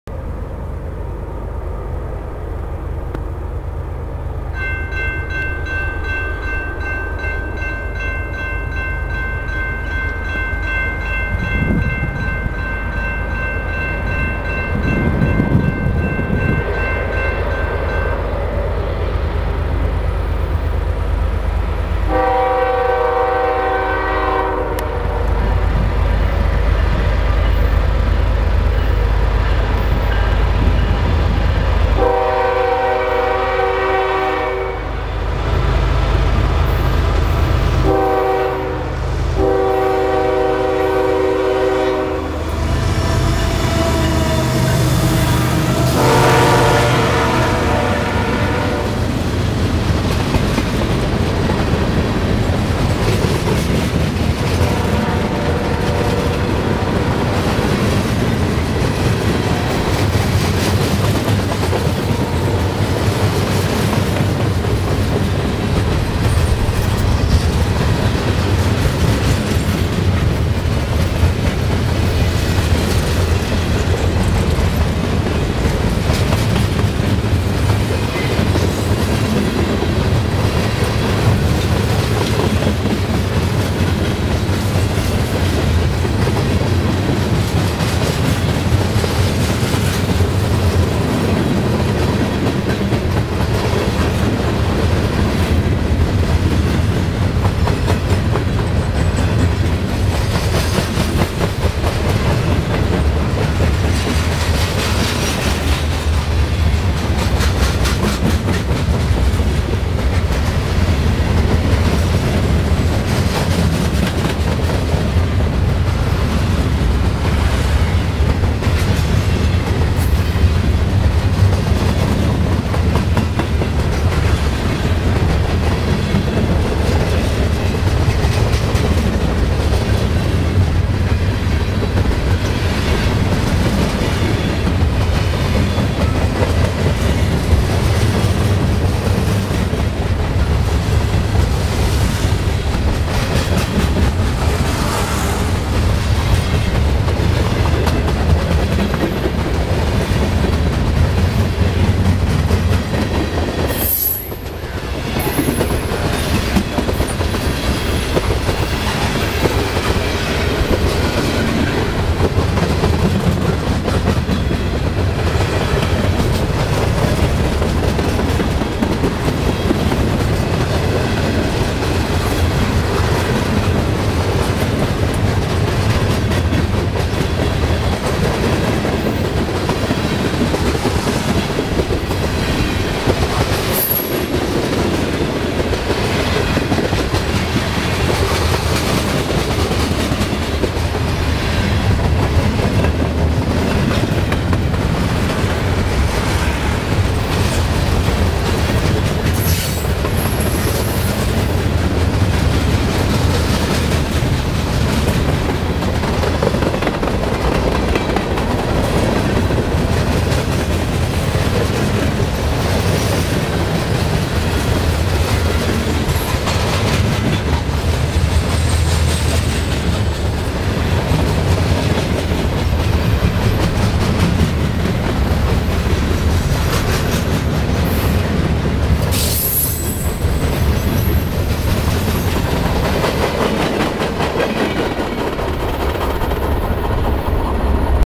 CP Coil Train East Chesterton IN
A CP Coil train heads East toward Elkhart IN.